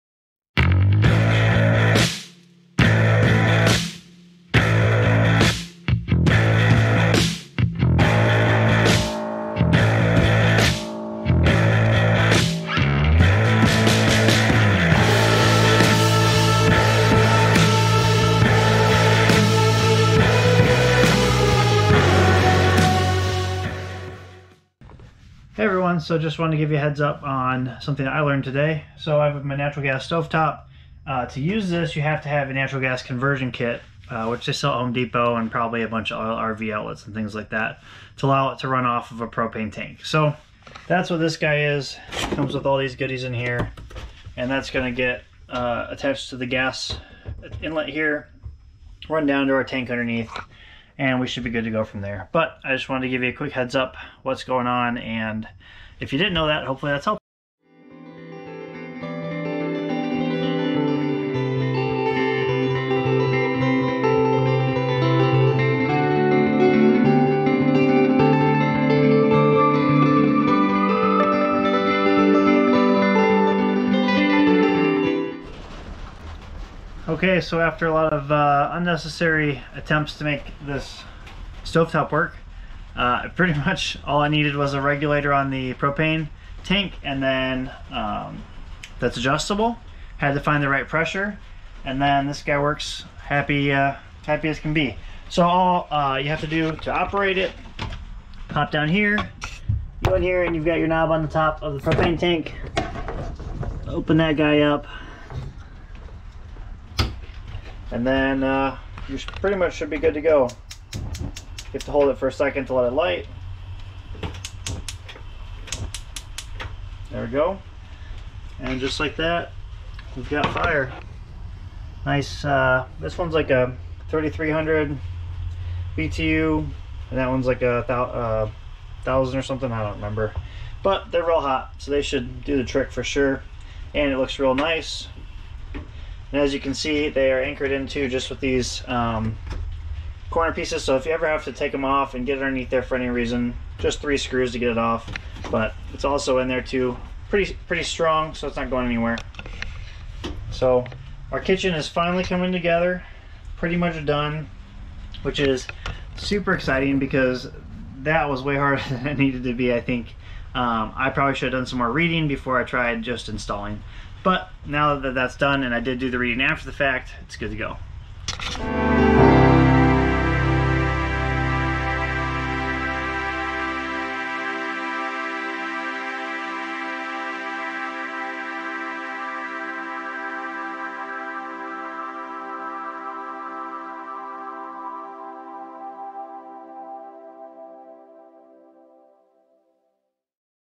S1E63 WE HAVE FIRE!!!!
Installing a propane stove I originally thought was natural gas.  So I make it look harder than it is.